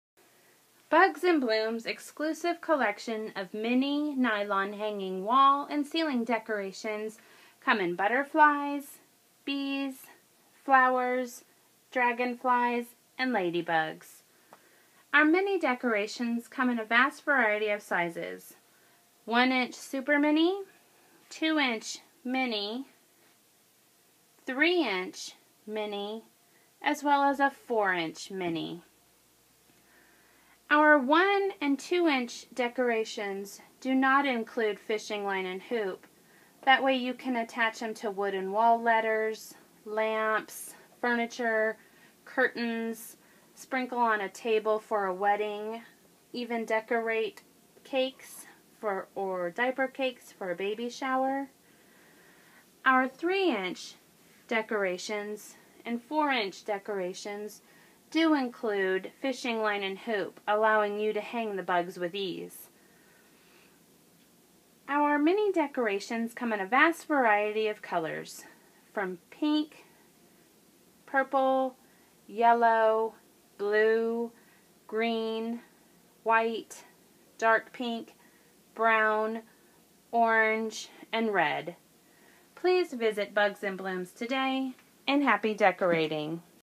Bugs-n-Blooms Mini Nylon Wall Ceiling Craft Party Decor : Butterfly Dragonfly Flower Ladybug Bee
are perfect to hang on any wall, ceiling, attach to lamps, furniture, decorate wedding tables, birthday cakes, or decorate a baby shower which later can be used in a babies nursery or girls garden themed bedroom.  Mini nylon decorations comes in butterflies, dragonflies, flowers, ladybugs, and bees. Some mini craft and room decor comes in various sizes and colors and pre-strung with fishing line and hoop to hang with ease.